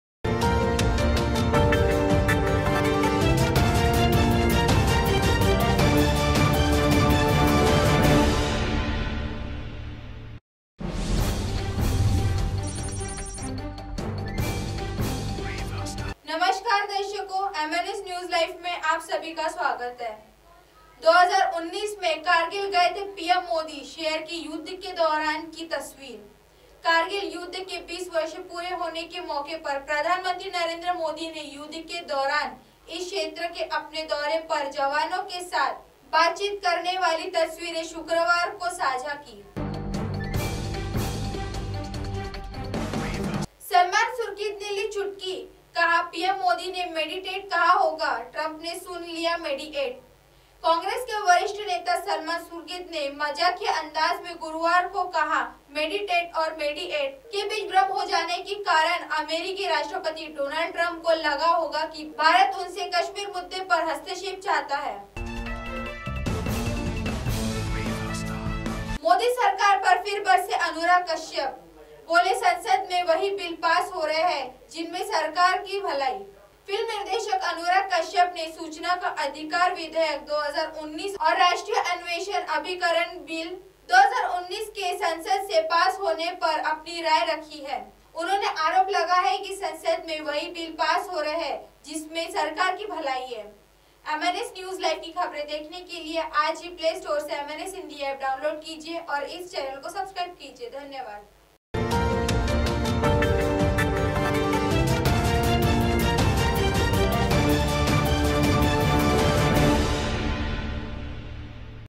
MNS #१९९९ में कारगिल गए थे पि ऍम मोदी, शेयर की युद्ध की दौरान की तस्वीर
MNSNEWSLIVE MNS #१९९९ में कारगिल गए थे पि ऍम मोदी, शेयर की युद्ध की दौरान की तस्वीर